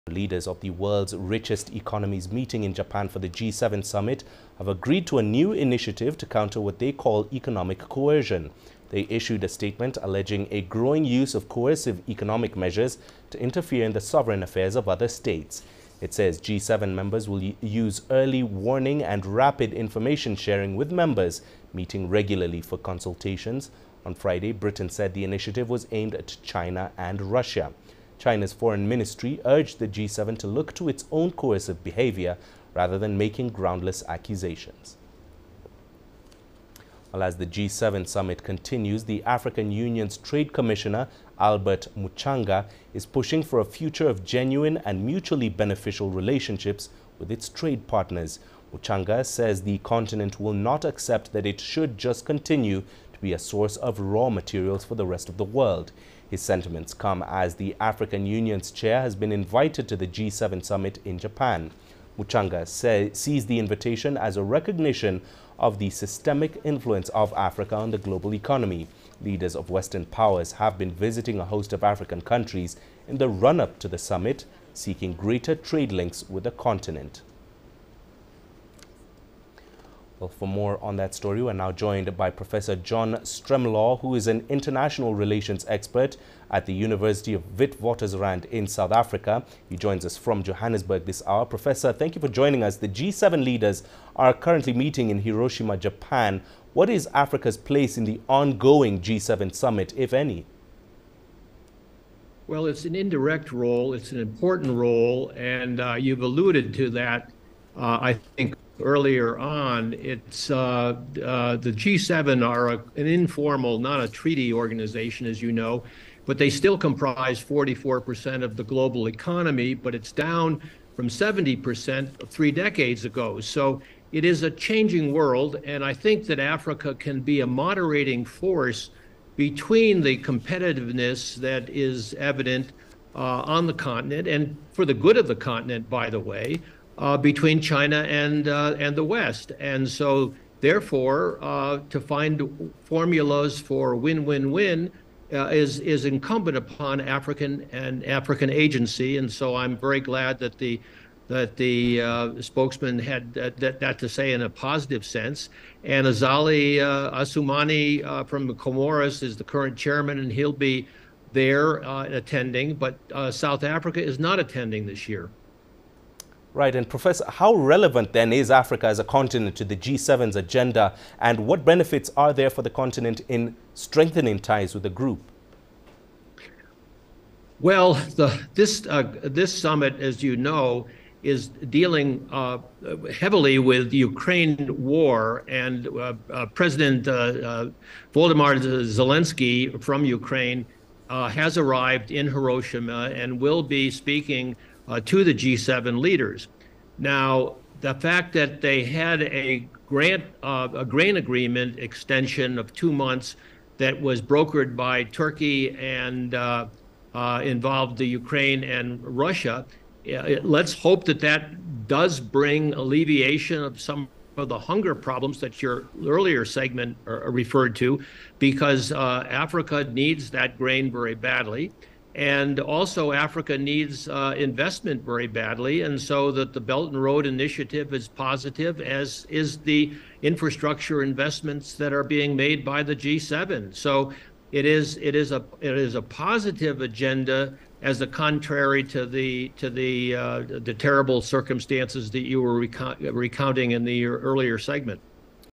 Africa’s relevance to the G7's agenda
Leaders of the world's richest economies met in Japan's Hiroshima for the G7 Summit. Professor John Stremlau, International Relations Expert, University of Witwatersrand in South Africa talks about Africa’s place in the ongoing G7 summit.